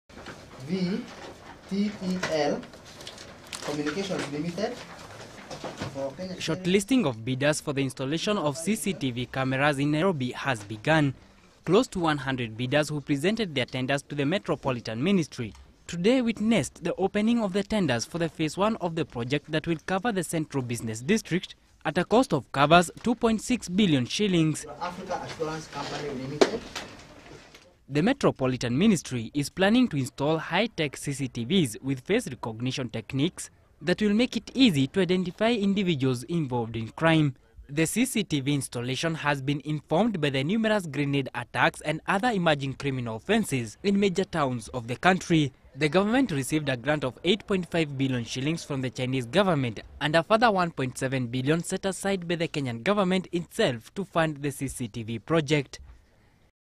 CCTV INSTALLATION
Shortlisting of bidders for the installation of CCTV cameras in Nairobi has begun. Close to 100 bidders who presented their tenders to the metropolitan ministry today witnessed the opening of the tenders for the phase one of the project that will cover the central business district at a cost of covers Sh2.6 billion.